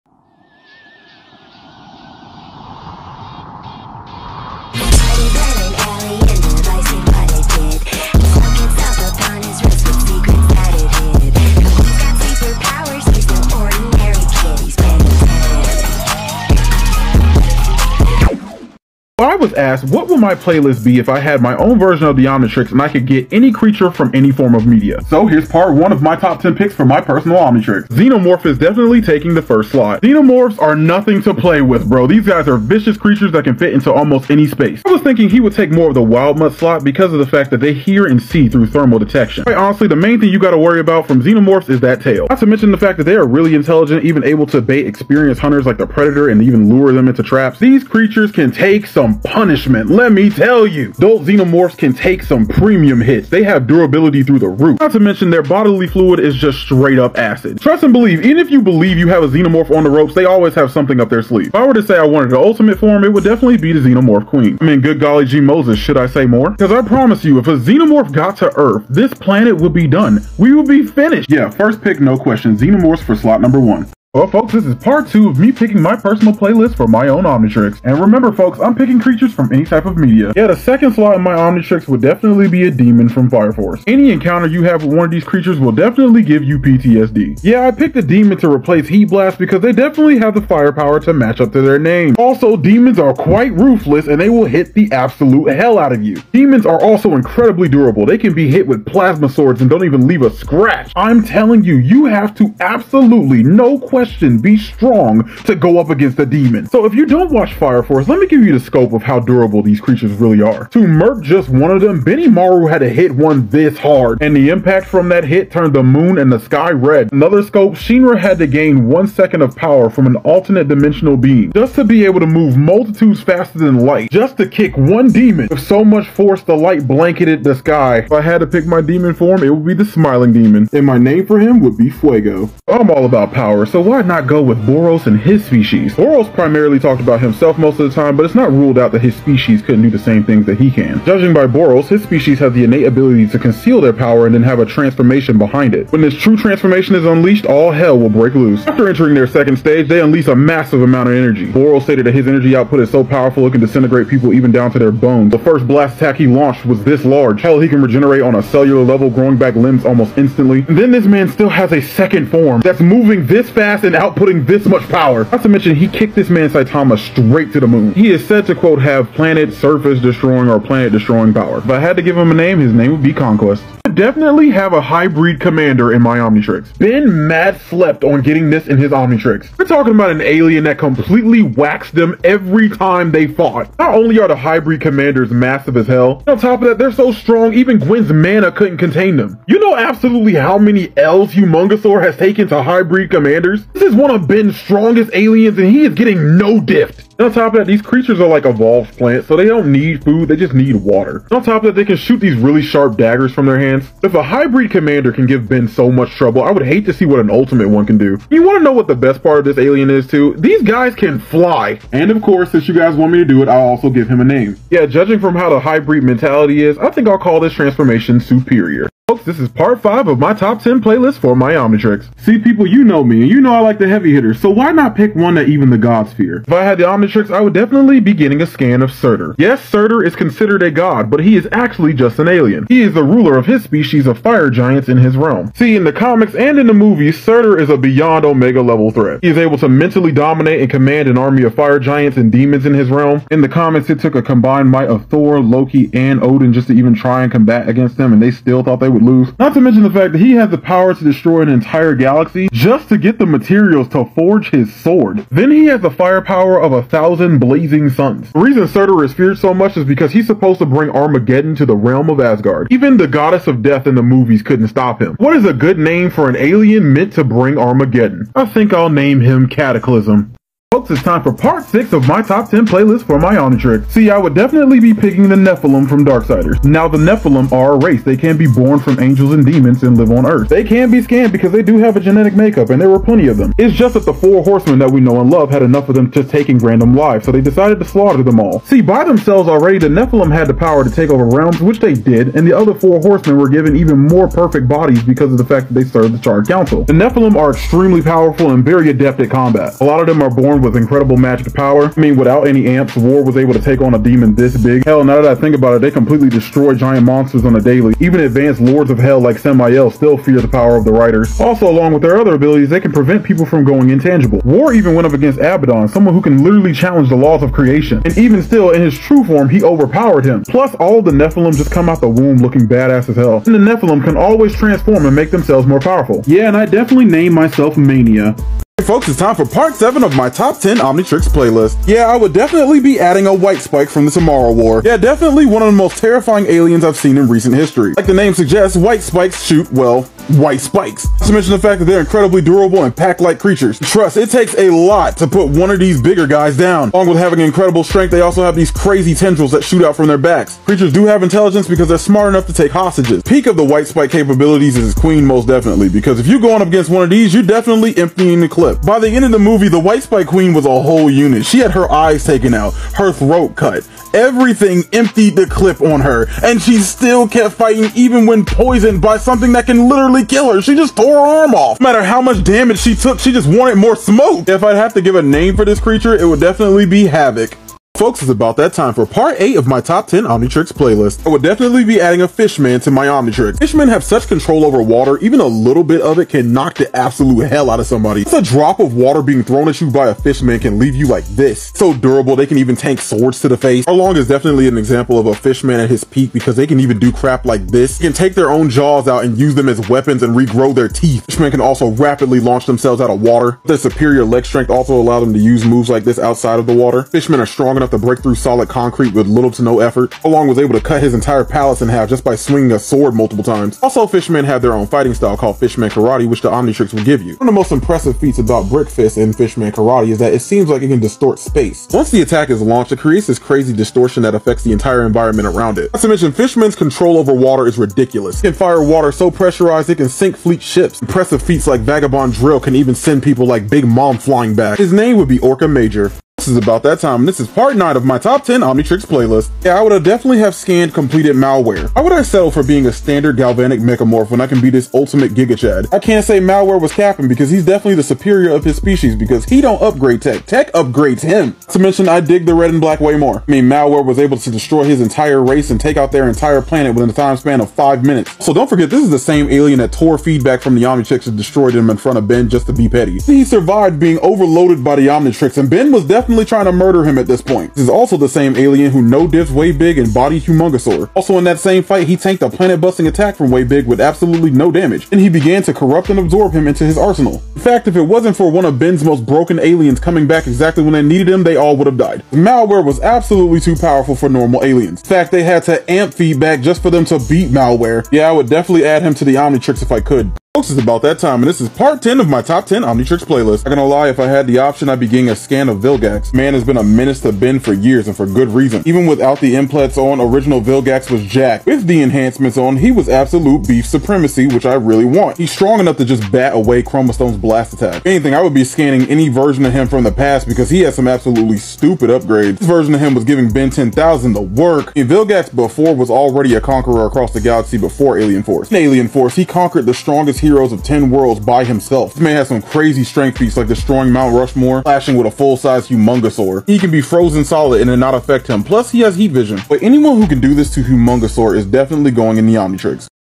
Top 10 Omnitrix Playlist Compliation
This is my Top 10 Omnitrix Playlist where I talk about creatures I would add to my own if I could. So if you love Ben 10 and love this Omnitrix series then drop a like for more content like this